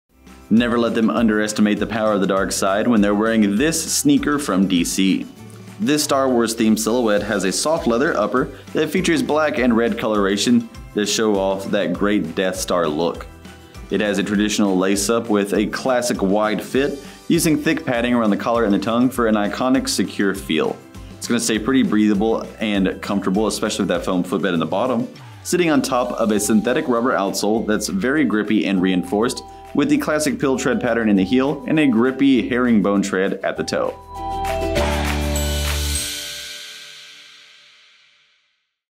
DC Kids Star Wars Court Graffik (Little Kid/Big Kid) SKU: 9666950
Get the super comfy and sporty DC&reg; Kids Star War&trade; Court Graffik Shoes and let your kids experience comfort all day long.
Leather upper.
Textile lining and insole.
 Lightweight mesh tongue.
Classic lace-up closure.
Round toe.
Foam padded tongue and collar for support.
Signature brand name logo on the tongue.
Pull loops for easy wear and removal.
Vent holes for breathability.
Collection: Star Wars&trade; DC Shoes Collaboration Collection.
Abrasion-resistant sticky rubber outsole featuring the brand's trademark PILL PATTERN&trade; tread pattern and herringbone tread.
Imported.